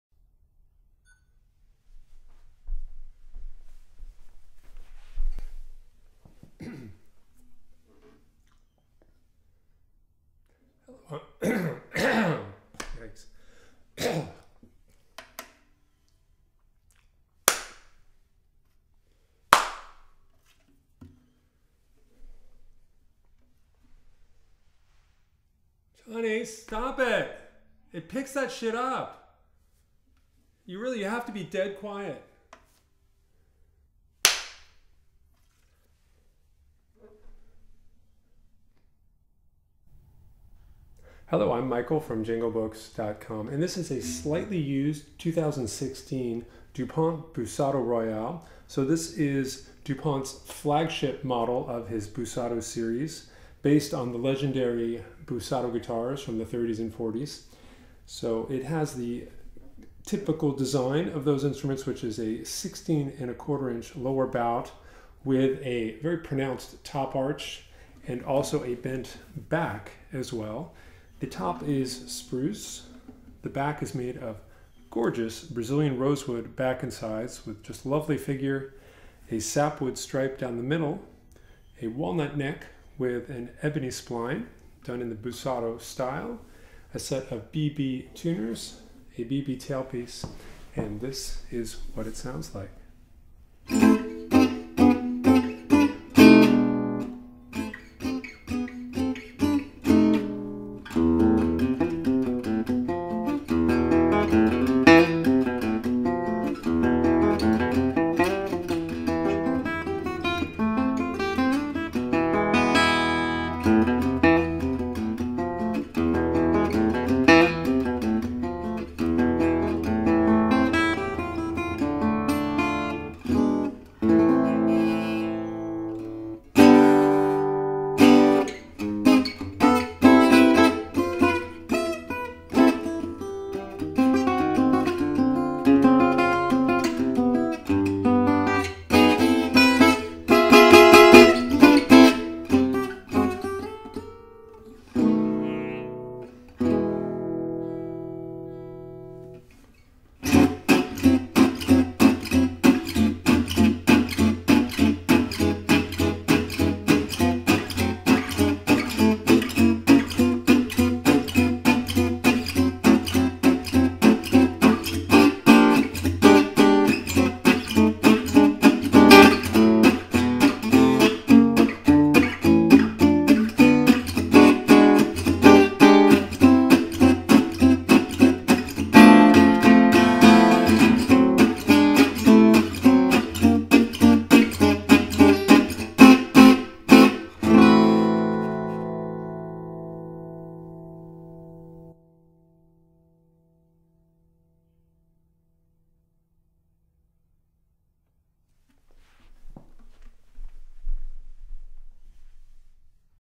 2016 Dupont Busato Royale Brazilian
This is Dupont's much anticipated copy of the classic Busato design. It is a near exact copy, with the classic Busato proportions: 670mm scale length and a 16 1/4" body size. It also features the famous Busato bombe (top arch) which is more curved then Favinos or Selmers. Like the originals, this guitar also has a beautifully arched back.

This guitar has a spruce top, gorgeous Brazilian rosewood back and sides, ebony binding, a three piece Walnut/Ebony neck, a vintage finish and deluxe Busato tuners.

The tone of this instrument is among the best of Dupont's Busato guitars I've had in the shop. In fact, the near perfect balance, exceptional projection, and soothing highs make it a clear candidate for one Dupont's best guitars period! Even for Dupont, this one is an outlier, with qualities normally only found in some of the rarest vintage instruments that are many times the price. It's only once every few years that I get a new instrument that is this good!

This lightly used example is in 100% perfect mint condition. It is indistinguishable from a new one making it a bargain at this price!